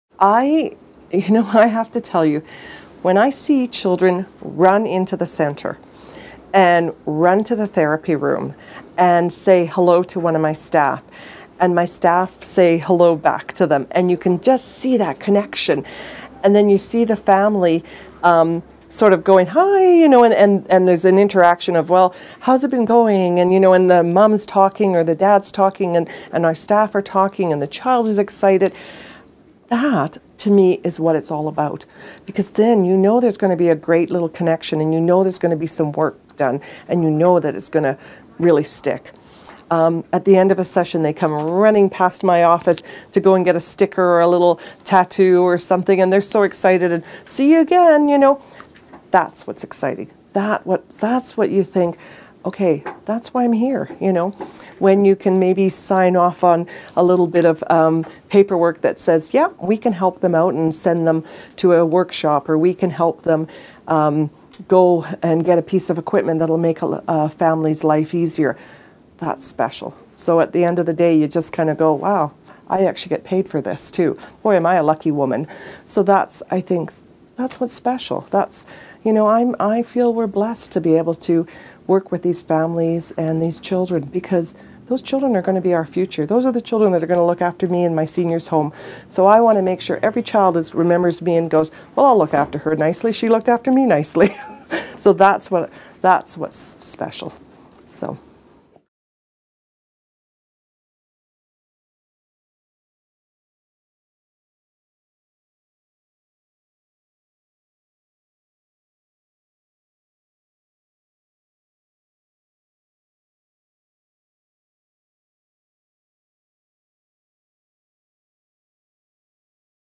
Mary-Ellen Everatt - What Is Special About The Work That You Do?
Mary-Ellen Everatt is the executive director at the Children’s Therapy & Family Resource Centre. Mary-Ellen loves seeing children run into the centre excited to see her staff. Making connections and establishing relationships with the families that use the therapy centre is what it's all about.